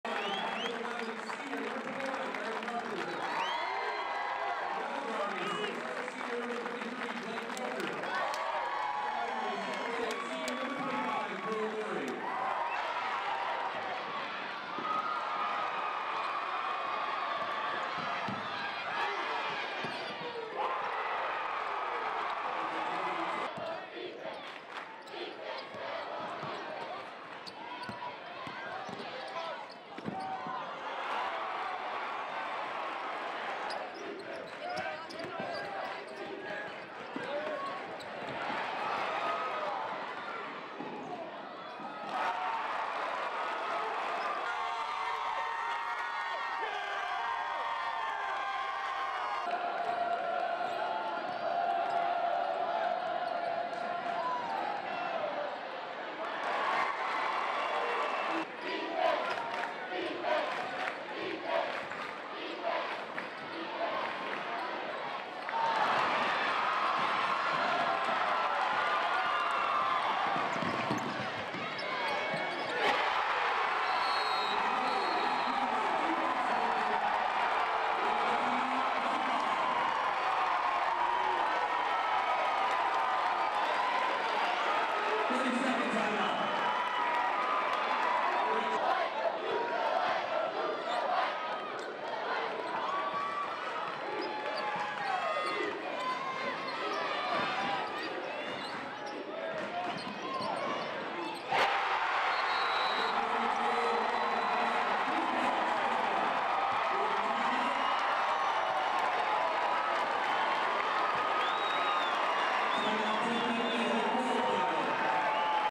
Web-Tipton 60 vs Delphi 48
Tipton knocks off Delphi for the 11th straight time and advances to the Regional finals.